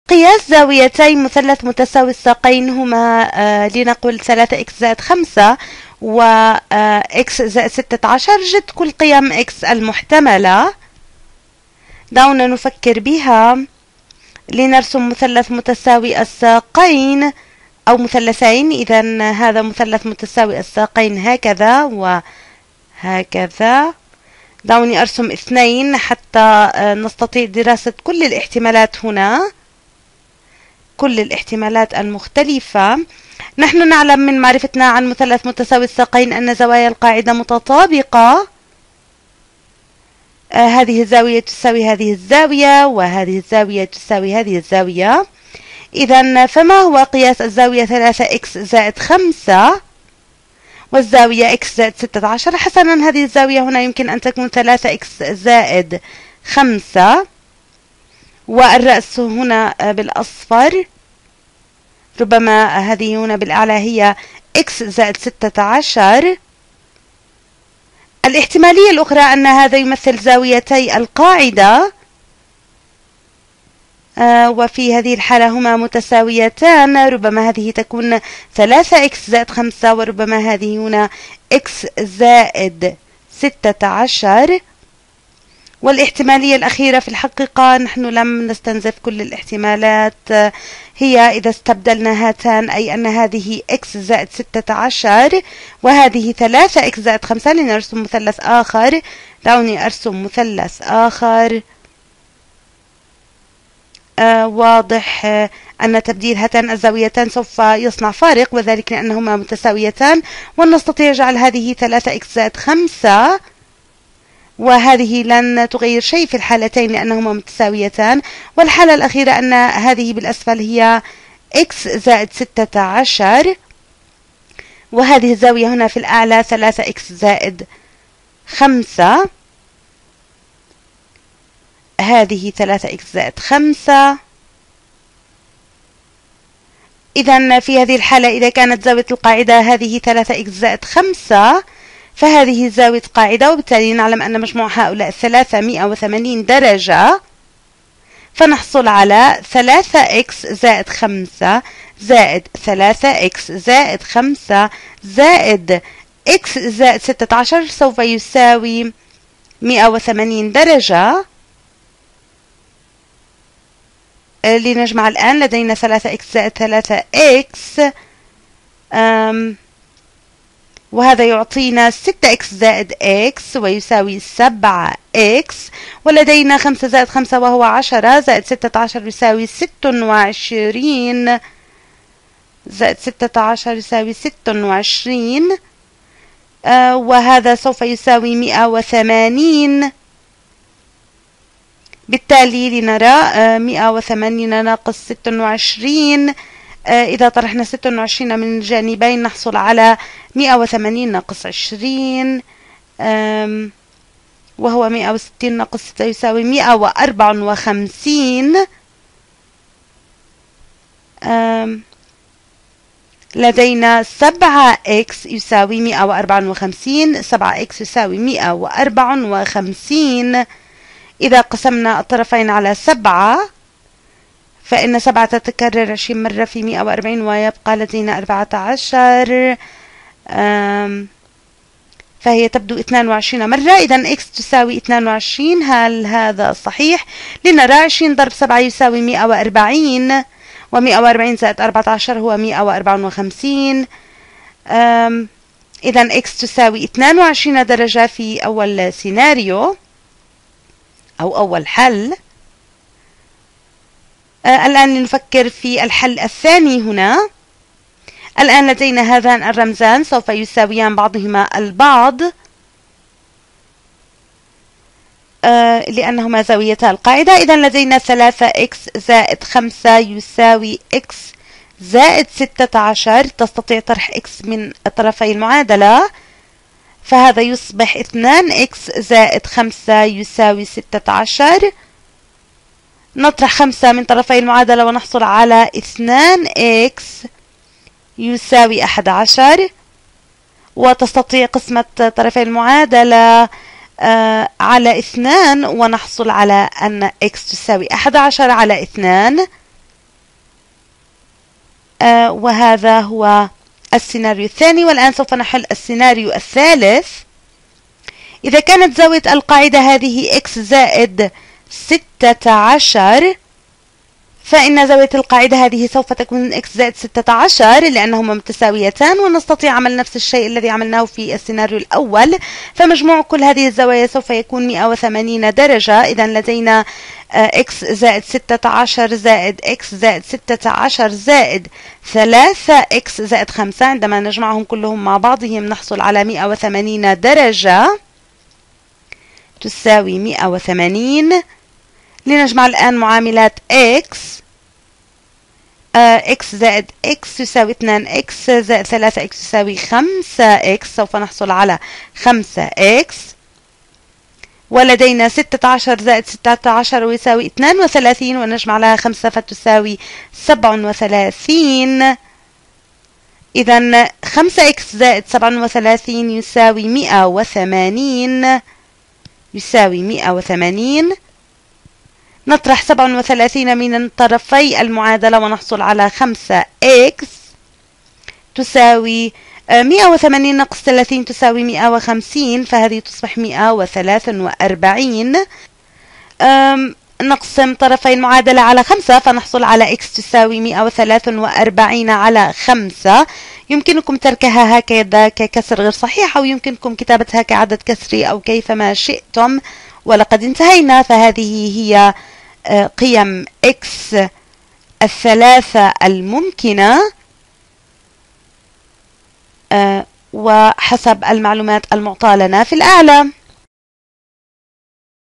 مسألة أخرى حول المثلث متساوي الساقين
Another Isosceles Example Problem
يتناول هذا الفيديو مسألة حول المثلث متساوي الساقين وحلها من خلال الخصائص الخاصة بهذا المثلث
شكر خاص لمؤسسة شركاء في التنمية المستدامة -- فلسطين